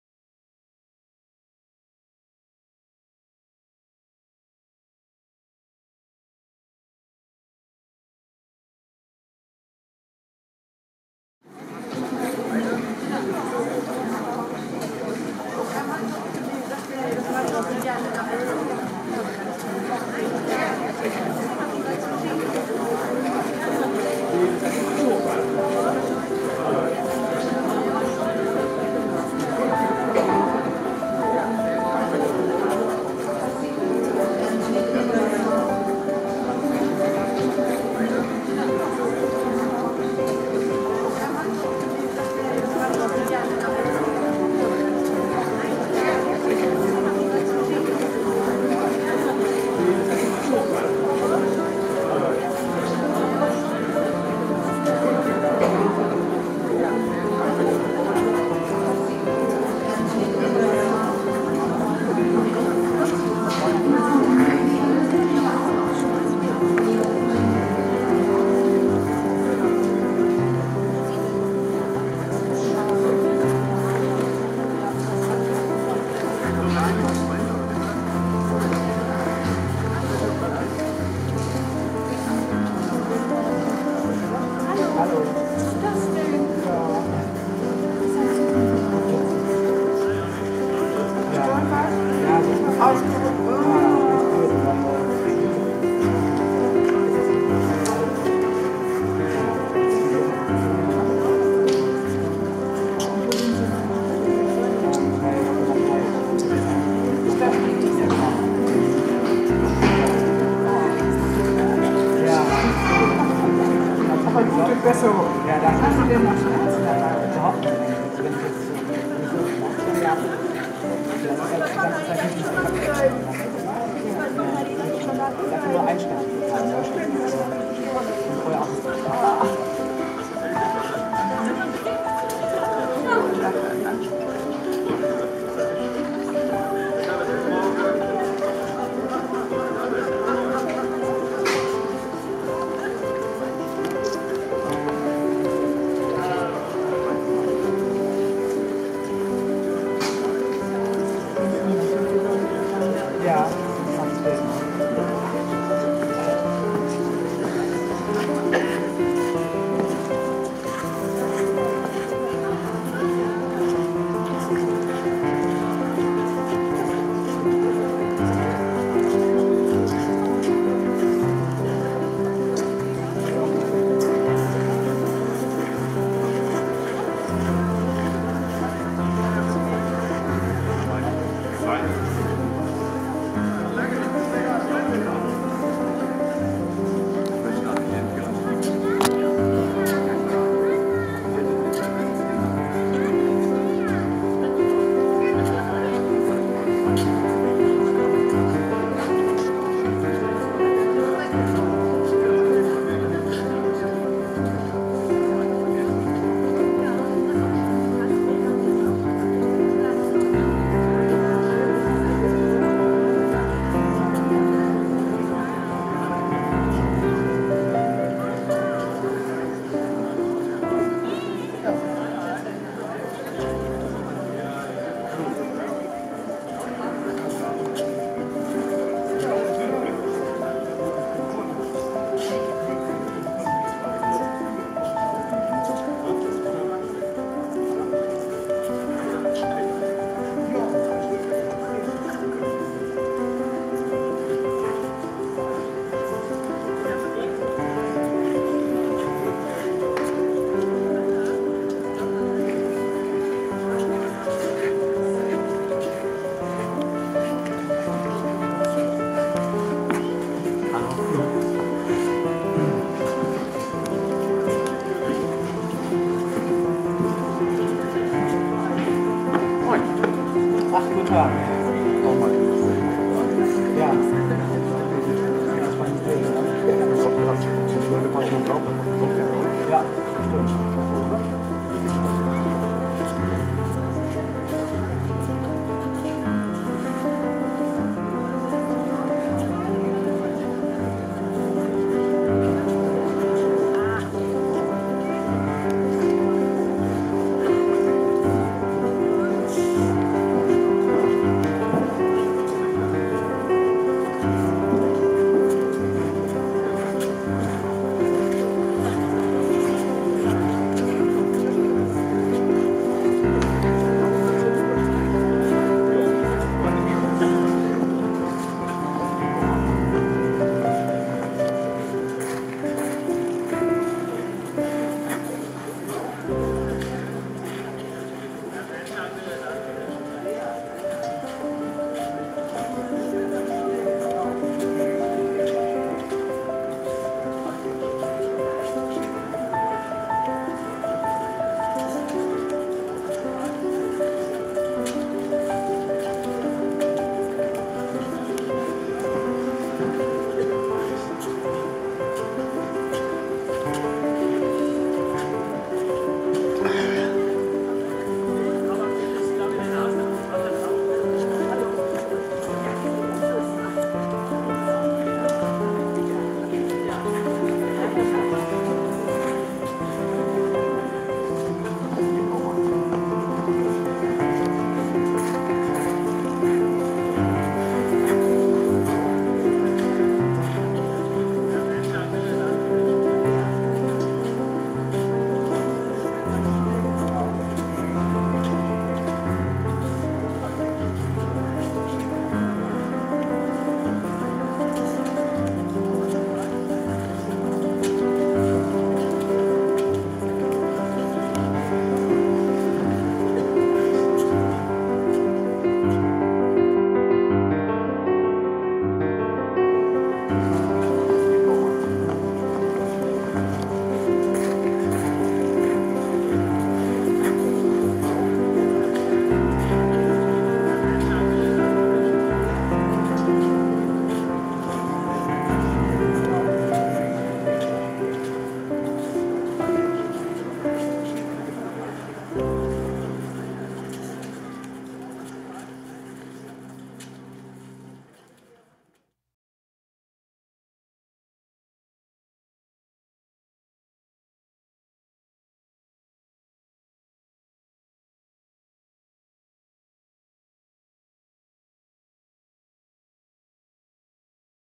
Labyrinth Kloster Frenswegen - Einweihung
Labyrinth Eröffnung  am 08. November 2015 Im Obstgarten des Klosters Frenswegen.